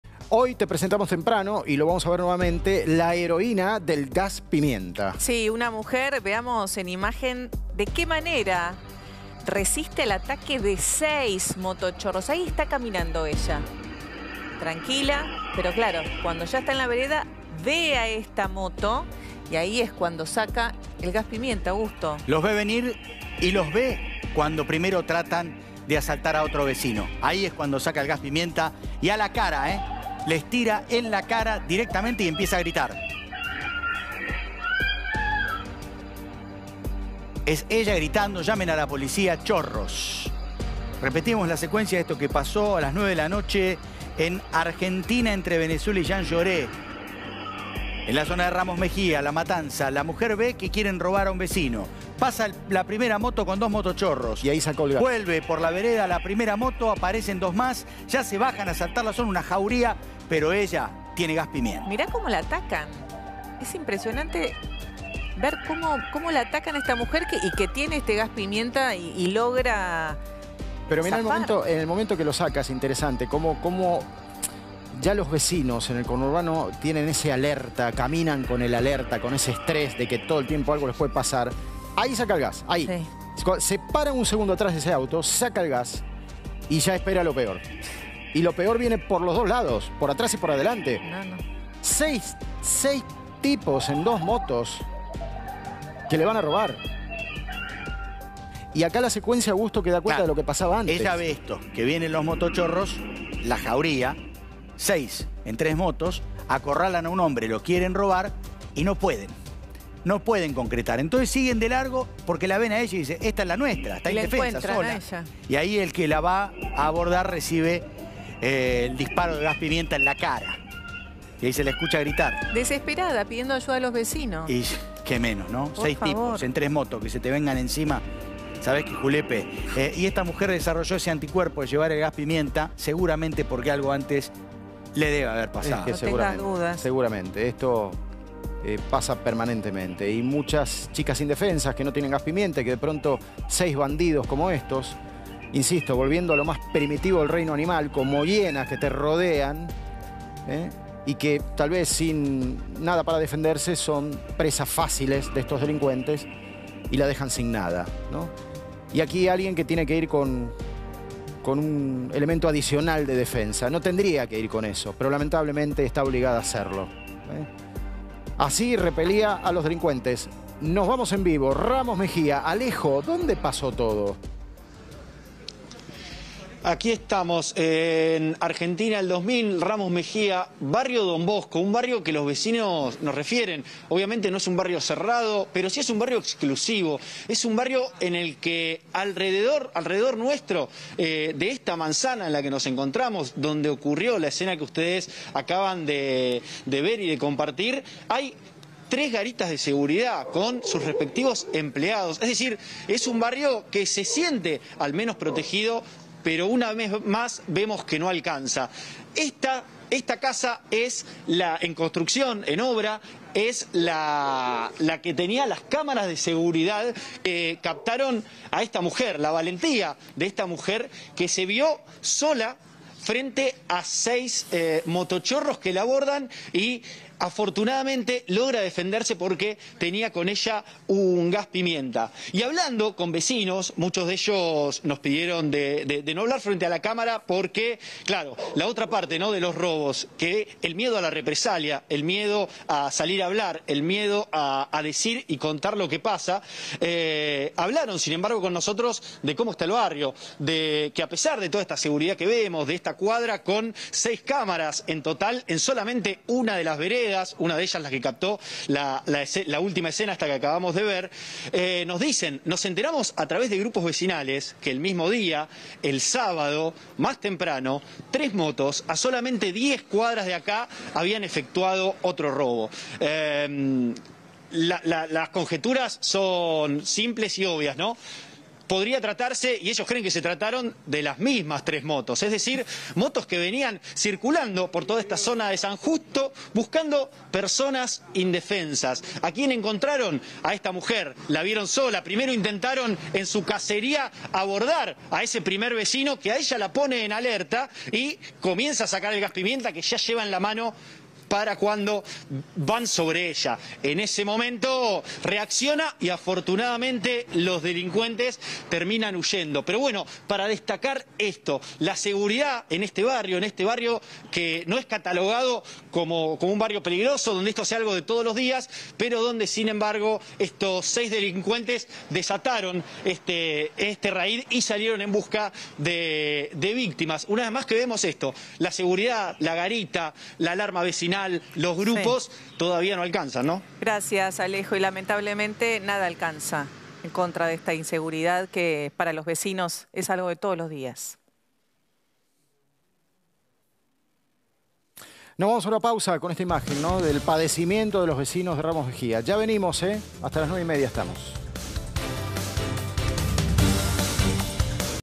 ECHÓ A SEIS MOTOCHORROS CON GAS PIMIENTA
Una mujer en Ramos Mejía se defendió de seis delincuentes con un "arma" de defensa personal que tenía en la cartera.